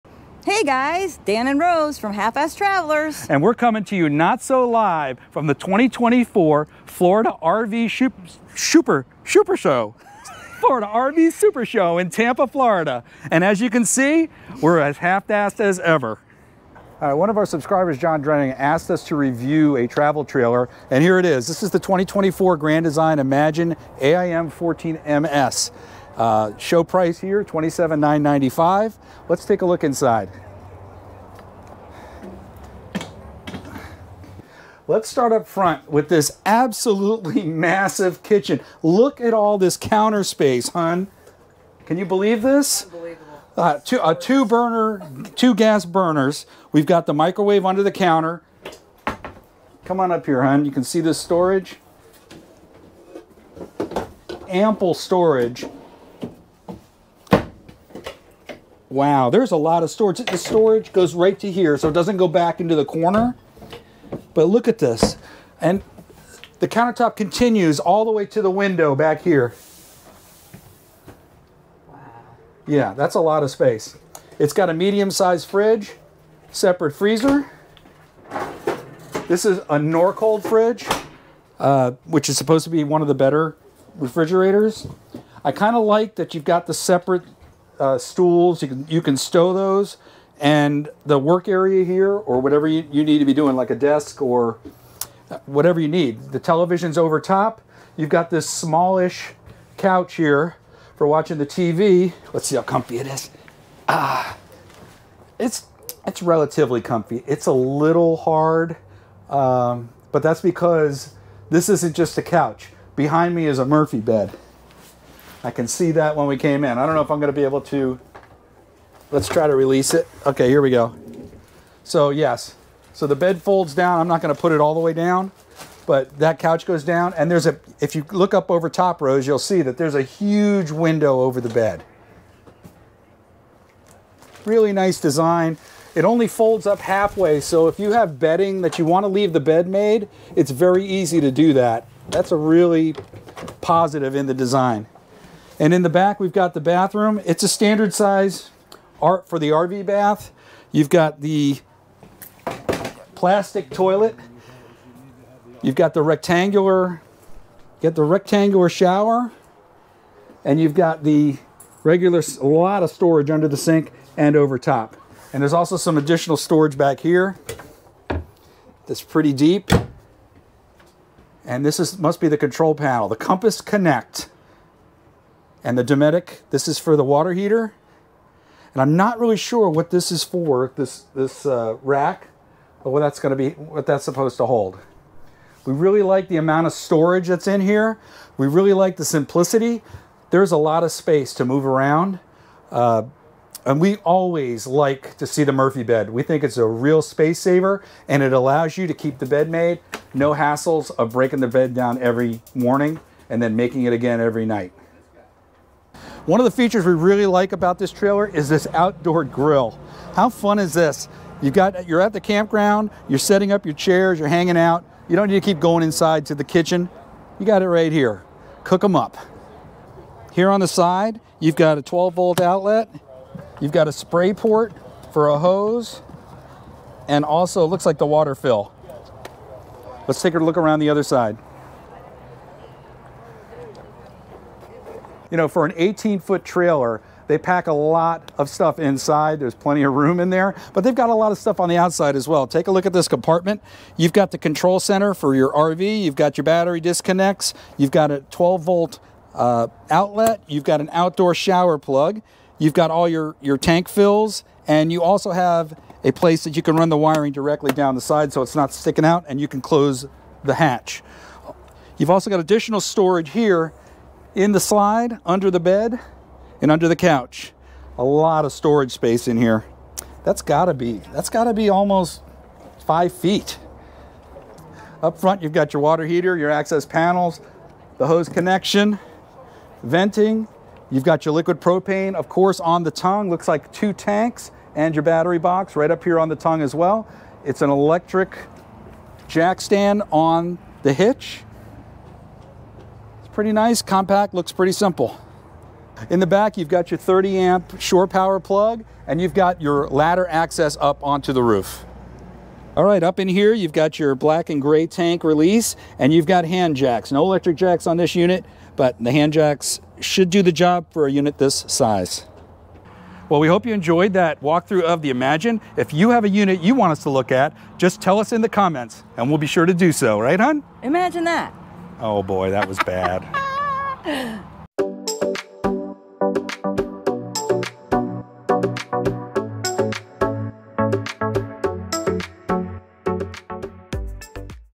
2024 Imagine AIM 14MS by Grand Design: Travel Trailer Walkthrough
We do a walkthrough of this travel trailer for one of our subscribers at the 2024 Florida RV Supershow.  The 2024 Grand Design Imagine AIM 14MS is a 17 foot plus travel trailer with a Murphy bed floorplan. The interior is roomy and there is a ton of counter space in the kitchen that includes a desk/bar.  There is a surprising amount of space in this compact trailer and the Murphy bed design allows for a couch in the living space as well.  Some of the features of this unit are an outdoor grill, electric awning, propane cooktop, electric refrigerator, and outdoor shower.  If you are looking for a medium sized travel trailer with an abundance of space and storage this may be your best choice in an RV.

You Tube: @halffasttravelers2702

Remember that everything takes twice as long when you're Half Fast.